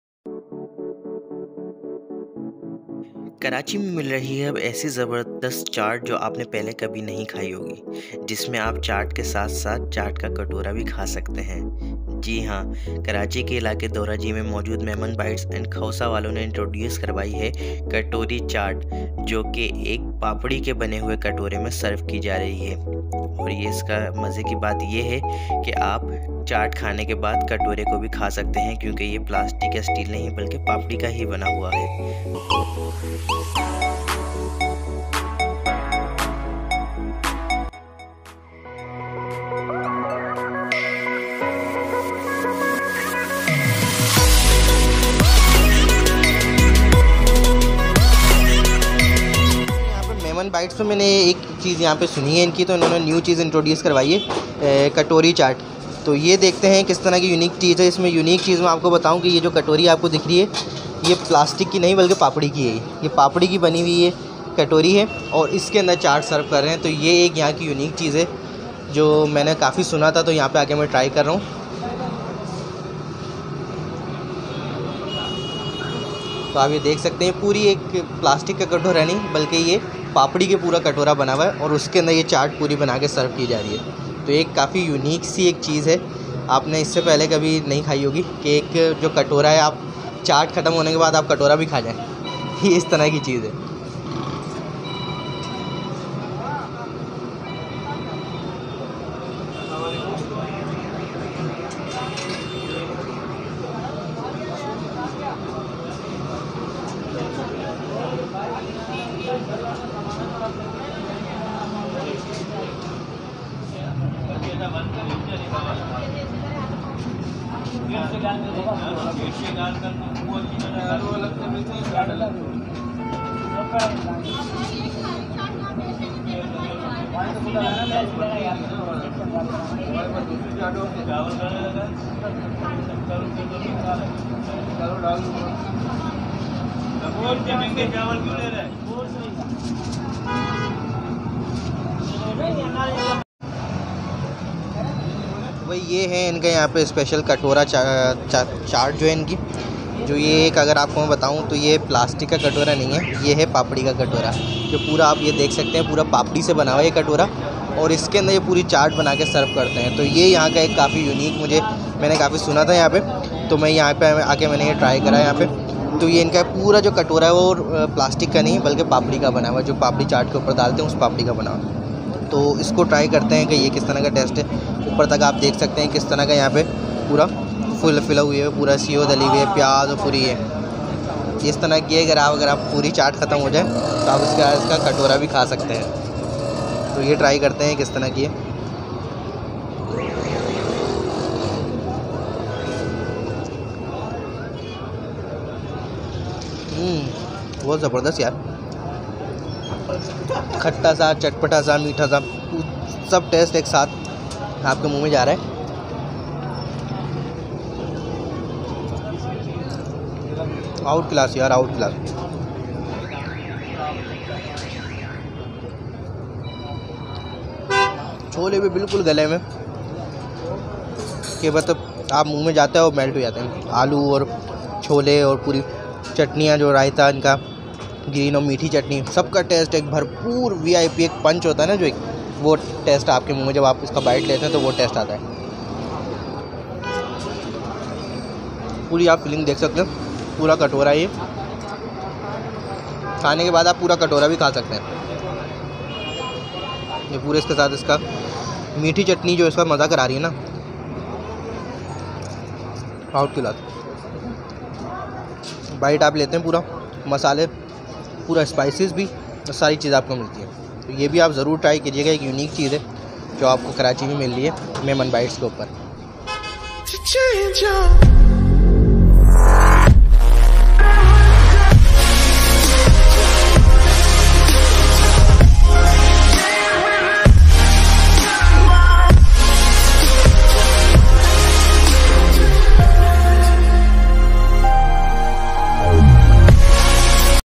Katori Chaat | Dhora Jee's Famous | Newly Introduce By Memon Bite & Khausa
New food vlog Exploring Street Food Karachi
exploring karachi street food its to much delicious and  softer in pocket
this Katori Chaat is most famous dish of Dhora jee karachi this thali have Chana chat , Dahi Barey , Khausa , Pani Puri Golgappey , Khattey aloo , and Khausa is most famous dish of Memon Bite  and also have Khatte aloo Chana,  Dhora jee is Chana chat food street and famous for gola ganda Chana chat chapati recipe,  Memon Khausa curry recipe, Indian style curry, Khatte chatpate aloo, Pani Puri Golgappey.
MEMON BITE KHAUSA
Full of Hygienic food I am recommend to all my viewers must try it.